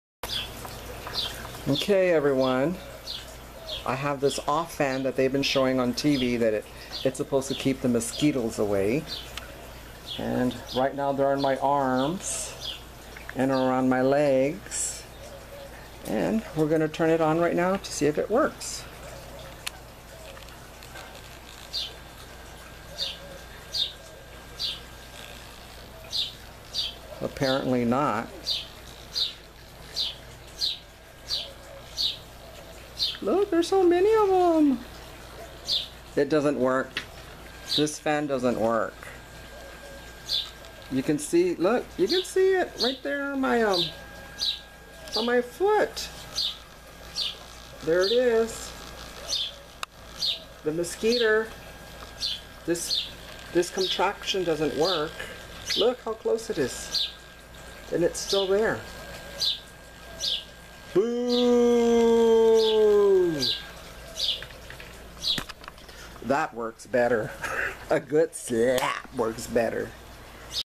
Off Mosquito Fan ( Test )
My friend Robert gave me This " Off Mosquito Fan "
he said it does not work ... So I decided to test it out
and share my results OUCH !!!
A Good old slap works better .
Does Not work .

This video ws made for The Springtime65 Show
Sa PATV